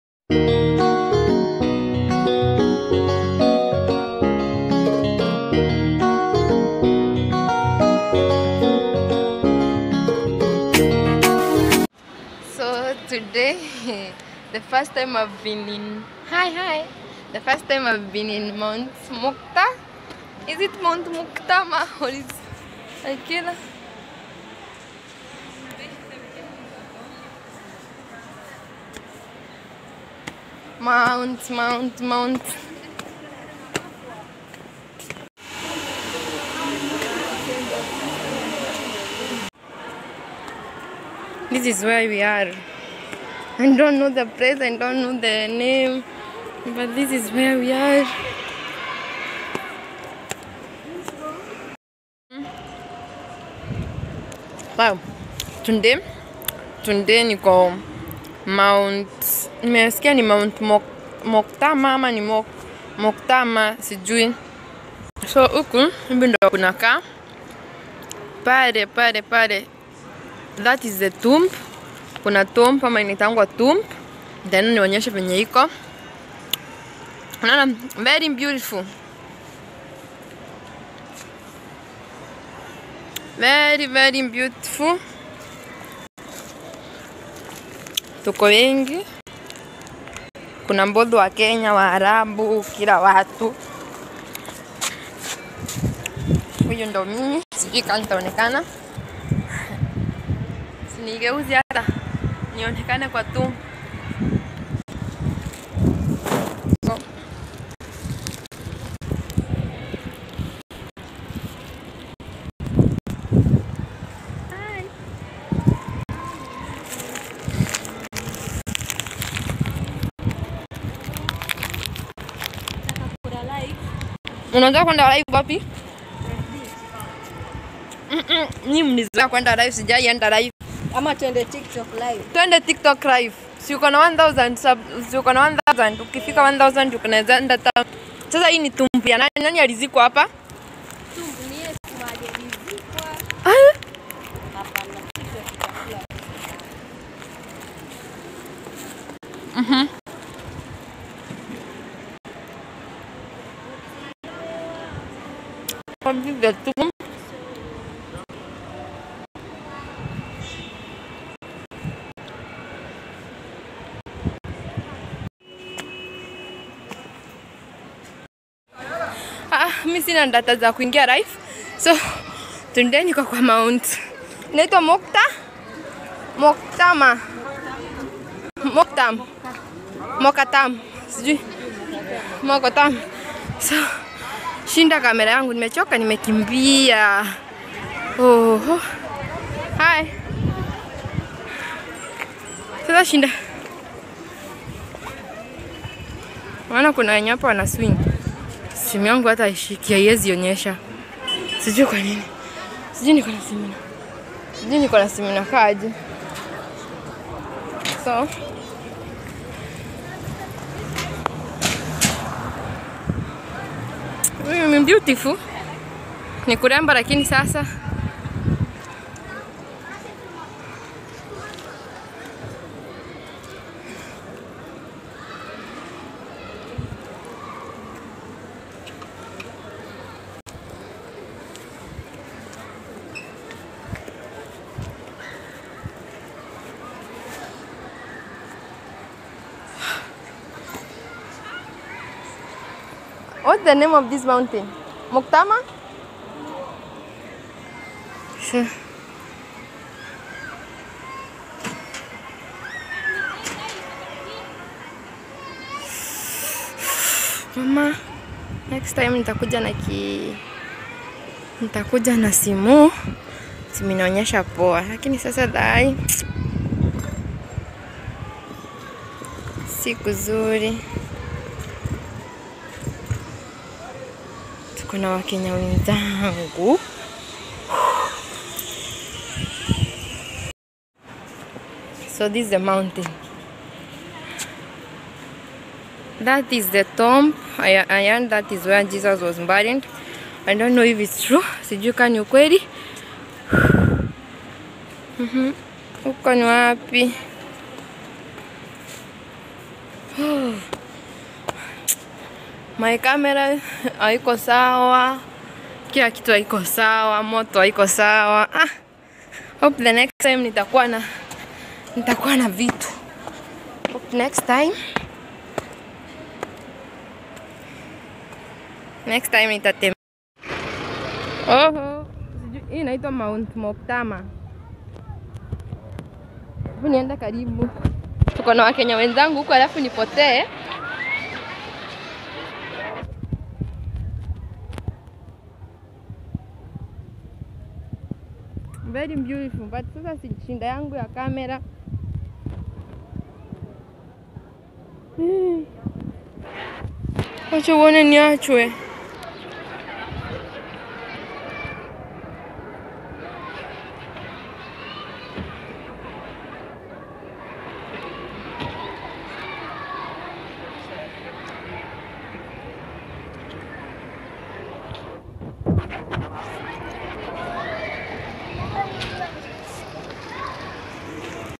visit to CAVE CHURCH//mt mokattam in Cairo Egypt 🇪🇬
a visit to mount mokattam 😂 it was the best day ...though I had not planned but I enjoyed everything. while taking the video I took only to share with my close friends but let me share with my YouTube friends

my battery was low kenyan in diaspora